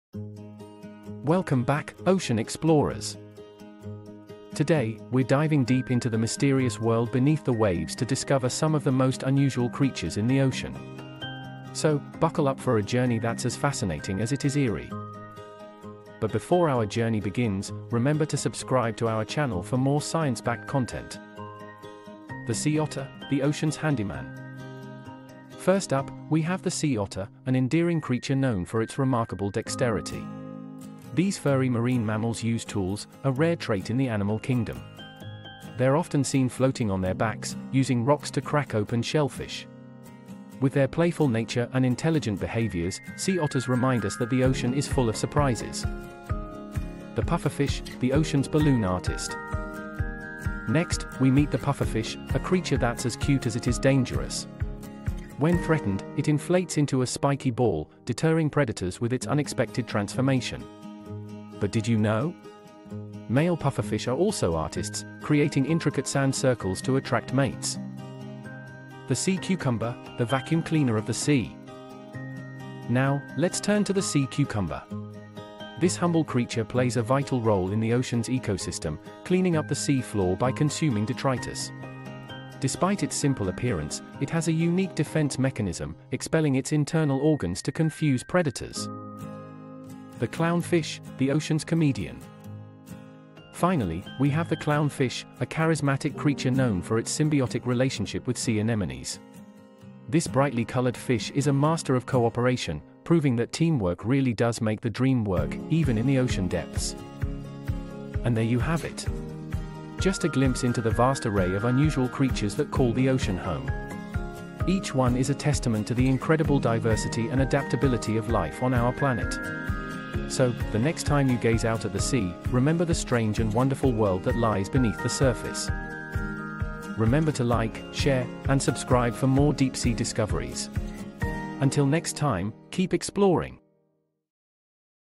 Most Unusual Creatures In The Ocean
Welcome to our journey through the vast ocean! 🌊🐠 In today's video, we're plunging into the depths to uncover some of the most unusual creatures in the ocean. From the tool-using Sea Otter to the artistic Pufferfish, the ocean is teeming with life that's as fascinating as it is diverse. 💙🔔

🔹 Time Stamps 🔹
0:00 - Introduction
0:15 - The Sea Otter: The Ocean's Handyman
0:45 - The Pufferfish: The Ocean's Balloon Artist
1:15 - The Sea Cucumber: The Vacuum Cleaner of the Sea
1:45 - The Clownfish: The Ocean's Comedian
2:15 - Conclusion

🔗 Links & Resources 🔗
Sea Otter Facts and Conservation:

Clownfish and Sea Anemones An Iconic Friendship:
(Credit: Nation Marine Sanctuary Foundation)